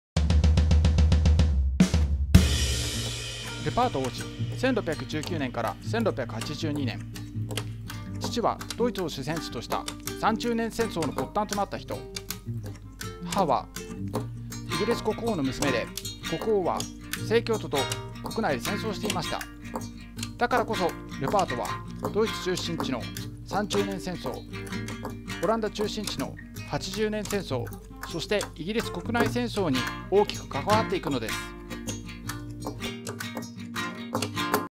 ルパート王子が3つの戦争と関わることになった出自
ルパート王子は、イギリス国内戦争においてボーイという犬を率いてイギリス国王チャールズ1世のもと戦い大活躍します。
そんなルパート王子は実は30年戦争、80年戦争にも関わっています。
3つの戦争に関わることになった出自について話します。